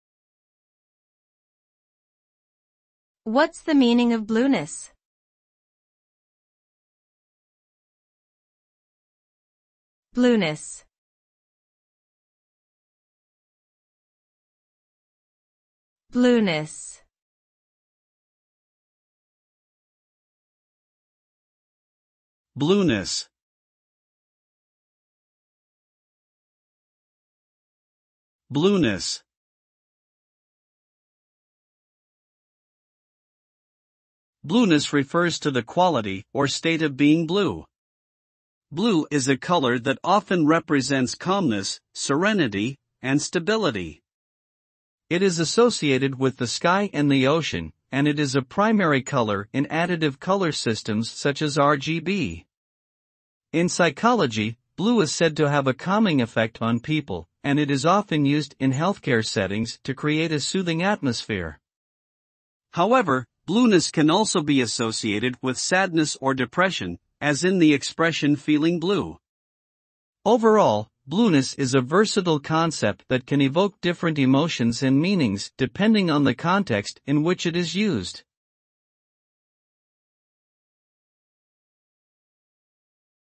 What's the meaning of "blueness", How to pronounce blueness?
This video tells you what "blueness" means, how to pronounce it, what is "blueness" definition, how to say blueness, provides a pronunciation guide and an accurate definition.
00:00 Pronunciation
00:32 Meaning